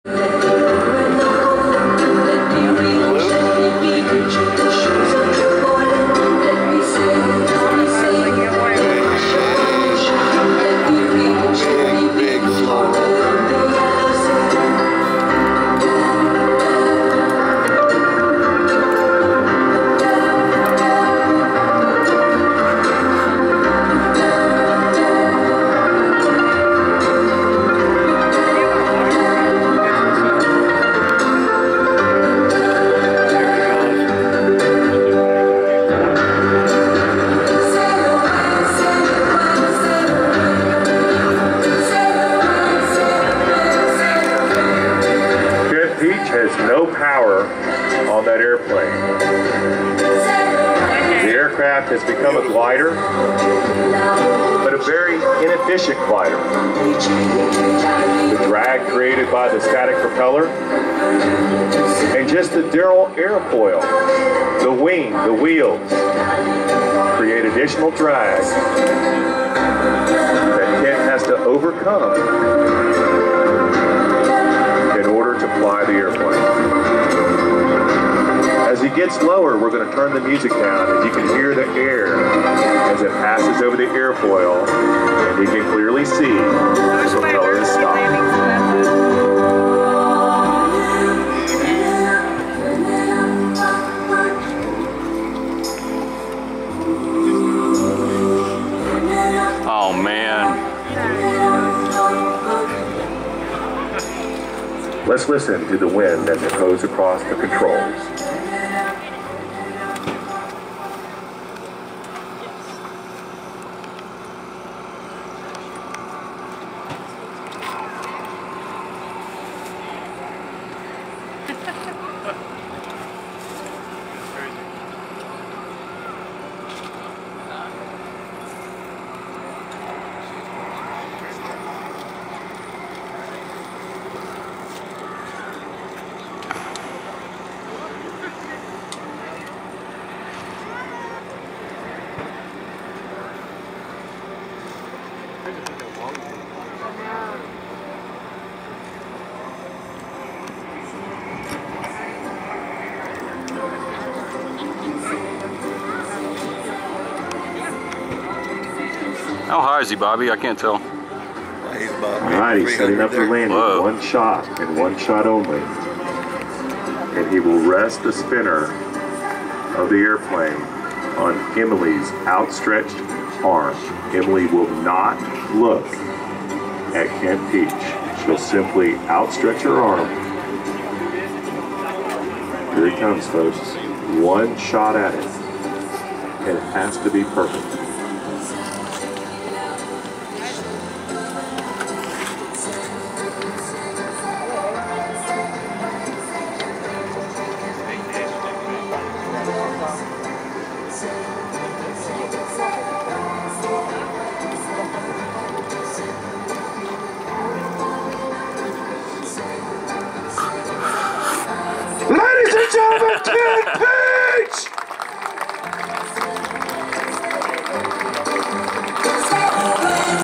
Kent Pietsch Dead Stick Routine - Heart of Texas 2015
Kent Pietsch in one of three routines stealing the Heart of Texas Airshow.
Dead stick from 6000'. Masterful energy management, amazing stick.